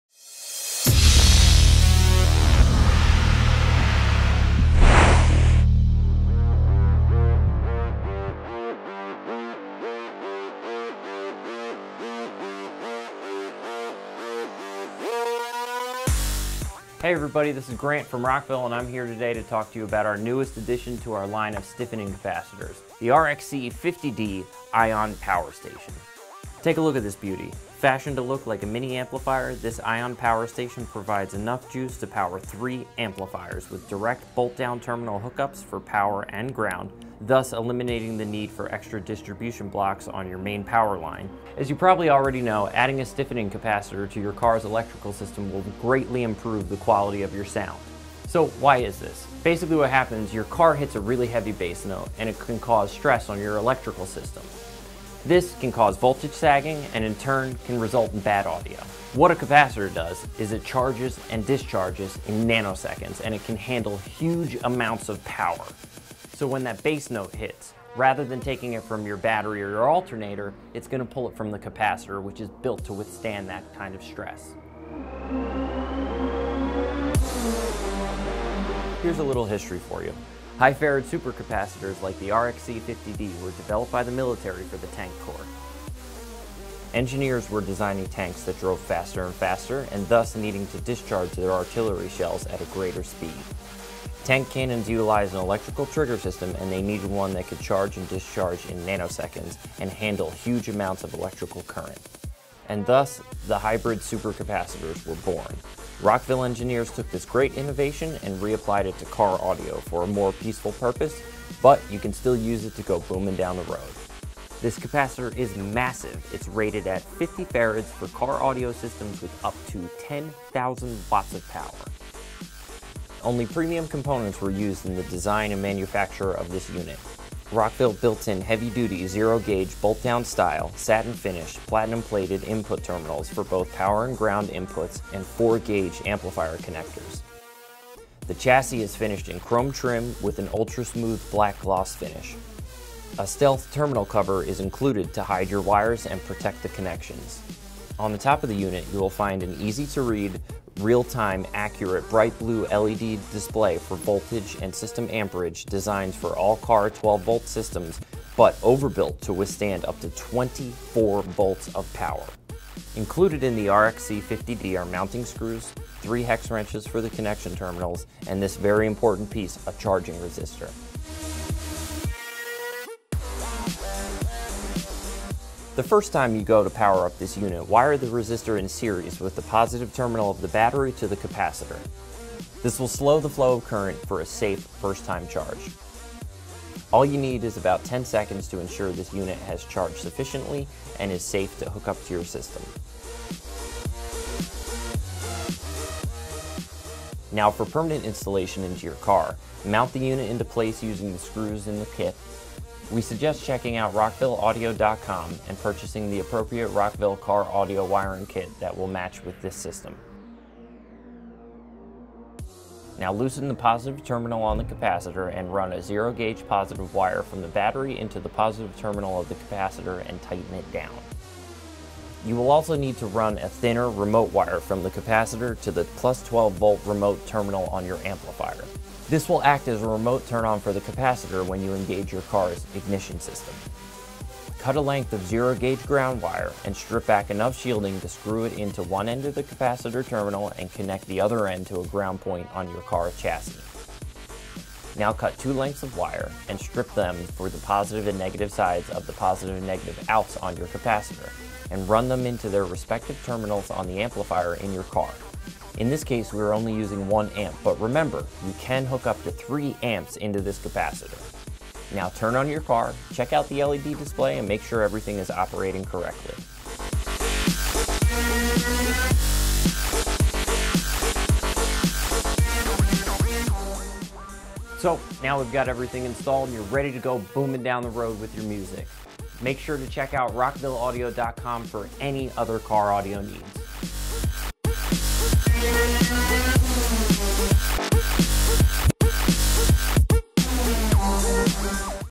Rockville RXC50D 50 Farad 10,000 Watt Extreme Ion Capacitor
Purchase on eBay

Purchase on Amazon

Whether your just adding a subwoofer or upgrading your entire sound system, it’s very easy to put an immense amount of stress onto your vehicles electrical system. High performance mobile audio systems require a vast amount of power, and let’s face it; cars are not made to handle the demands of today’s high performance audio system.

Dimming headlights is usually the first sign of an over-worked electrical system.

The Rockville RXC50D Capacitor provides an easy solution for your cars electrical strains. This competition grade capacitor reserves power on tap for your car amplifiers, and only delivers it as needed. This will ensure your car audio system gets all the power it needs without straining your vehicles electrical system.

The Rockville RXC50D is a 50-farad Ion capacitor and is capable of working with systems totaling over 10,000 watts of power. We used the latest in advanced Ion Capacitor technology to ensure that our capacitor not only holds more energy, but also delivers it in nano seconds; enabling the RXC50D to not only be faster, but also more efficient than traditional capacitors on the market. No short cuts were taken when designing the RXC50D; using bolt down style platinum plated terminals, heavy duty 0 gauge inputs for both power and ground, solid 4 gauge amplifier connectors, along with a beautiful polished chrome trim, topped with an ultra smooth gloss black finish.

The RXC50D was made for car audio enthusiasts, but can be used by anyone. With its bright and easy to read blue LED-lit display, you can accurately monitor your system status, ensuring proper voltage and amperage. The RXC50D supports up to 3 amplifiers and also has built in IC-Controlled circuitry which alerts you if your voltage is too high (+17.5v) or too low (-10v.)

Features:

50 Farad Hybrid Capacitor
(1) 0 Gauge and (3) 4 Gauge Input / Output Terminals with Stealth Removable Cover
Operating Voltage: 10 – 16V
Surge Voltage: 24V
Blue LED’s - *18.8 Voltage and Amperage Display
*Due to limitations of Digital Display, the maximum display readout is 18.8V, though the capacitor functions up to 24V.
Accurate live-time Voltage & Amperage monitoring
Custom Rockville Amplifier style housing with heavy duty platinum plated, bolt down terminals
IC-Controlled voltage protection circuitry
Polished Chrome trim with an Ultra smooth Gloss Black Finish
Stealth Removable protective terminal cover